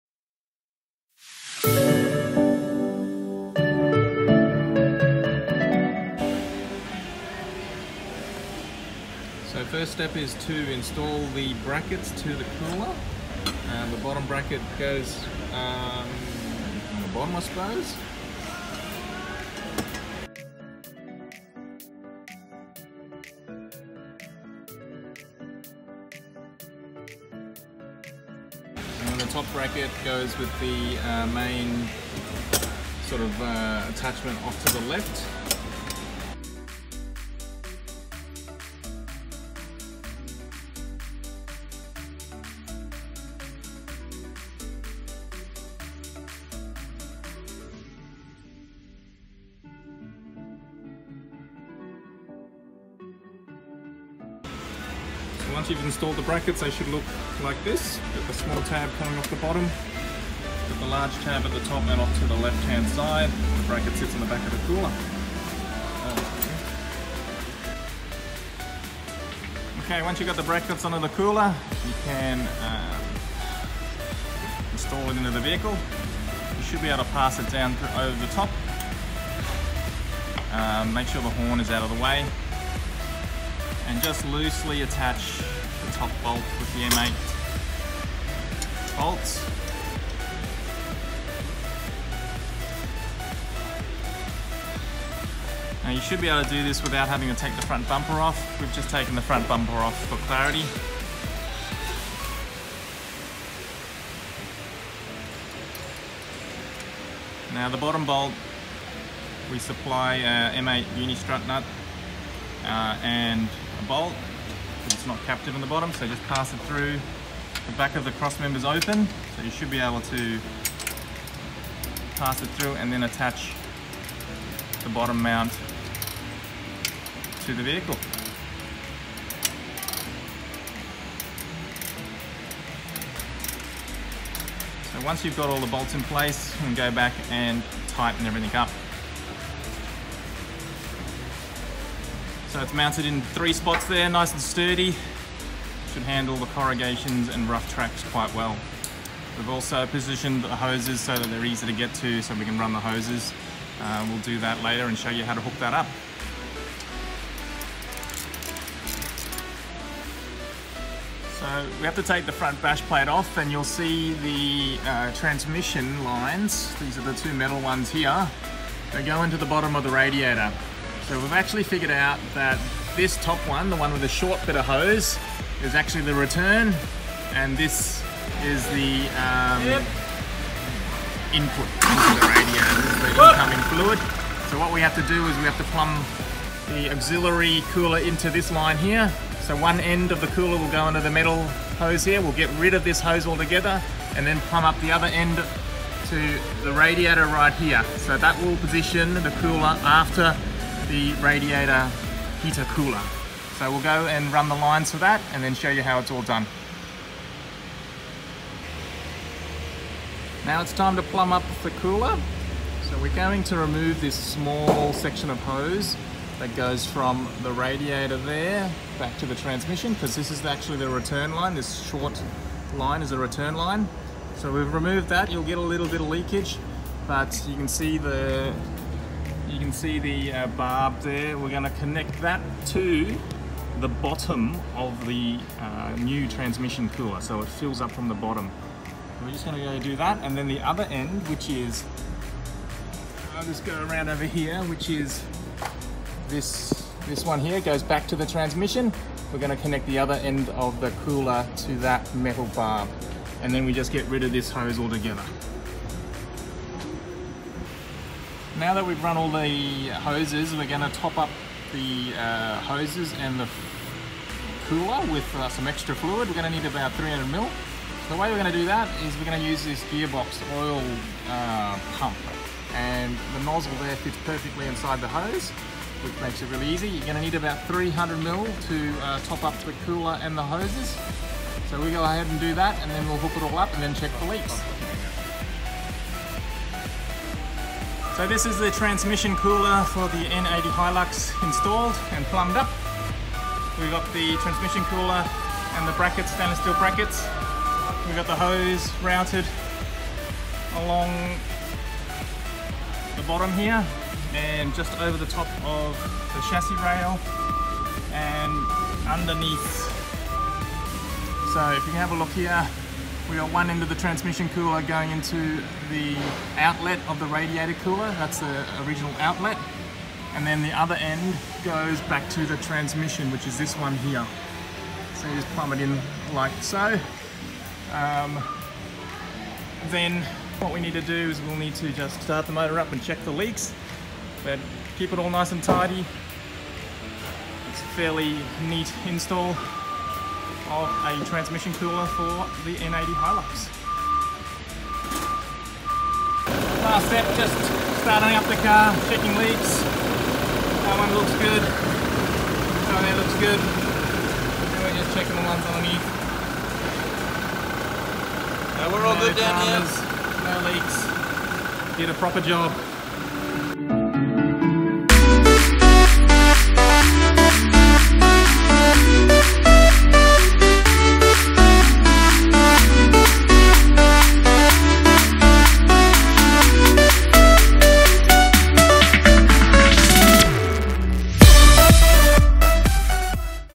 Hilux N80 KAON Automatic Transmission Cooler installation
Installation of KAON transmission cooler kit in N80 Hilux.
Kit provides extra cooling for towing and offroad driving.

Australian made Davies Craig aluminium cooler.
2mm Stainless steel brackets that attached to existing holes.
Easy DIY installation.